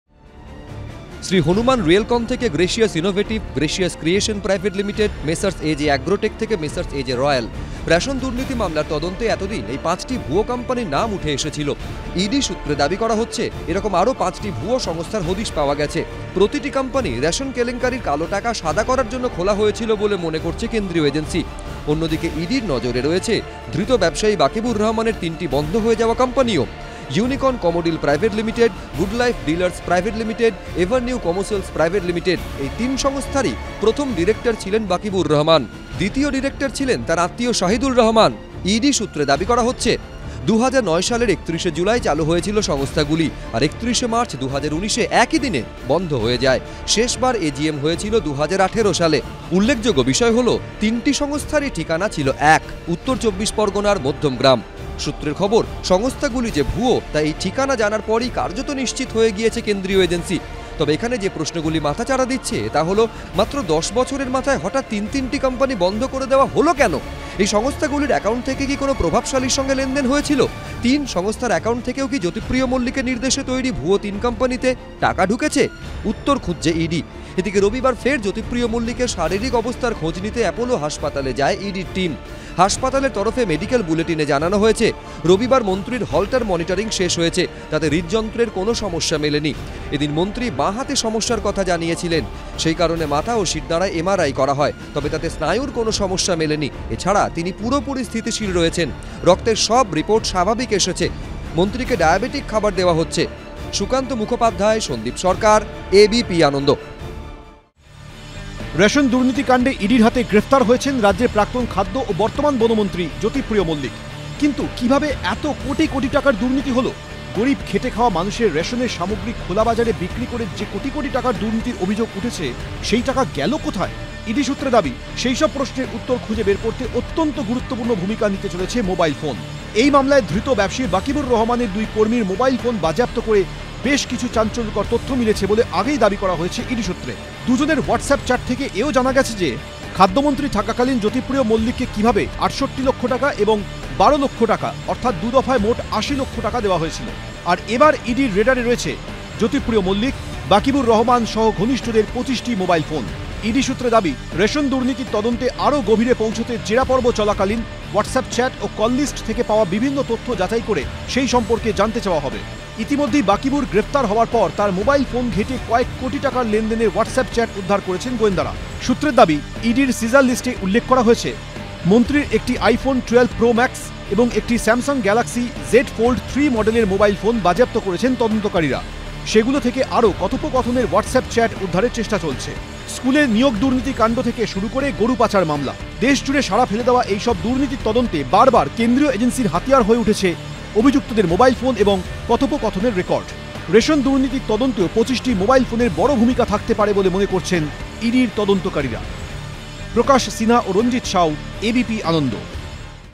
Jyotipriya Mallick Health:জেনারেল কেবিনে জ্যোতিপ্রিয় মল্লিক, আরও ৫ ভুয়ো সংস্থার হদিশ, খবর ED সূত্রে
ABP Ananda LIVE: ICU থেকে জেনারেল কেবিনে পাঠানো হল জ্যোতিপ্রিয় মল্লিককে (Jyotipriyo Mallick Health Status)। তিনি এখন স্থিতিশীল রয়েছেন বলে জানিয়েছে অ্যাপোলো কর্তৃপক্ষ। এদিকে রেশন দুর্নীতির (Ration Scam Investigation) তদন্তে আরও ৫টি ভুয়ো সংস্থার হদিশ পাওয়া গেছে বলে ED সূ্ত্রে (ED On Ration Scam) দাবি করা হচ্ছে। নজরে রয়েছে বাকিবুরের বন্ধ হয়ে যাওয়া ৩টি সংস্থাও (Shell Companies Found)।